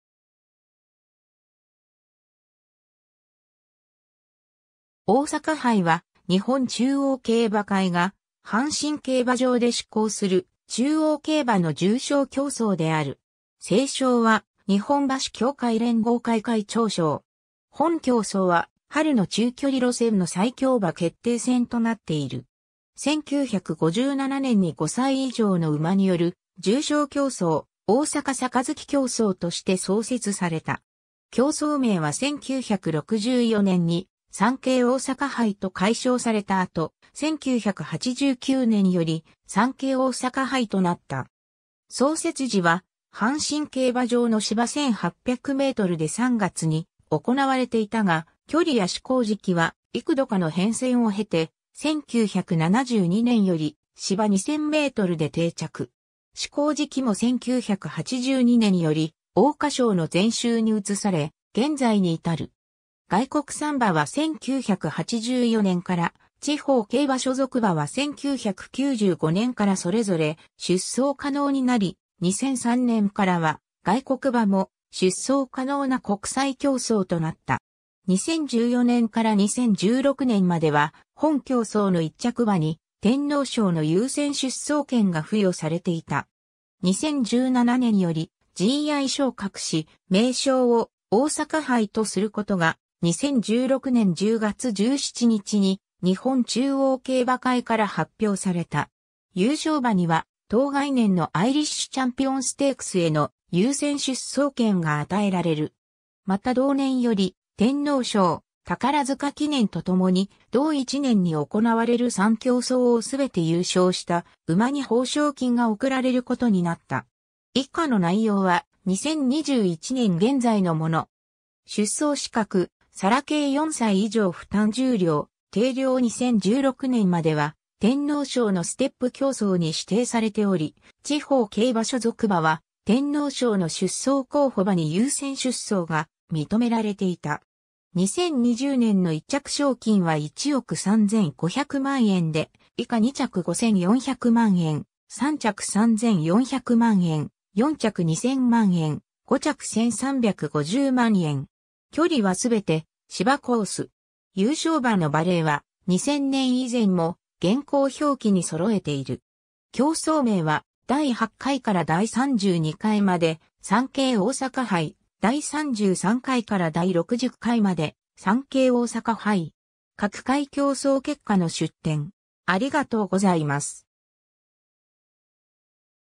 大阪杯
大阪杯（おおさかはい）は、日本中央競馬会（JRA）が阪神競馬場で施行する中央競馬の重賞競走（GI）である。
 正賞は日本馬主協会連合会会長賞。
 本競走は、春の中距離路線の最強馬決定戦となっている。
 1957年に5歳（現4歳）以上の馬による重賞競走「大阪盃競走」として創設された。
競走名は1964年に「サンケイ大阪杯」と改称された後、1989年より「産経大阪杯」となった。
 創設時は阪神競馬場の芝1800mで3月に行われていたが、距離や施行時期は幾度かの変遷を経て、1972年より芝2000mで定着。
施行時期も1982年より桜花賞の前週に移され、現在に至る。
 外国産馬は1984年から、地方競馬所属馬は1995年からそれぞれ出走可能になり、2003年からは外国馬も出走可能な国際競走となった。
2014年から2016年までは本競走の1着馬に天皇賞（春）の優先出走権が付与されていた。
 2017年よりGI昇格し、名称を「大阪杯」とすることが、2016年10月17日に日本中央競馬会から発表された。
優勝馬には当該年のアイリッシュチャンピオンステークスへの優先出走権が与えられる。
また同年より天皇賞（春）、宝塚記念とともに同一年に行われる3競走を全て優勝した馬に褒賞金が贈られることになった。
 以下の内容は、2021年現在のもの。
 出走資格：サラ系4歳以上 負担重量：定量（57kg、牝馬2kg減） 2016年までは天皇賞（春）のステップ競走に指定されており、地方競馬所属馬は天皇賞（春）の出走候補馬（2頭まで）に優先出走が認められていた。
 2020年の1着賞金は1億3500万円で、以下2着5400万円、3着3400万円、4着2000万円、5着1350万円。
 距離はすべて芝コース。
 優勝馬の馬齢は、2000年以前も現行表記に揃えている。
 競走名は第8回から第32回まで「サンケイ大阪杯」、第33回から第60回まで「産経大阪杯」。
 各回競走結果の出典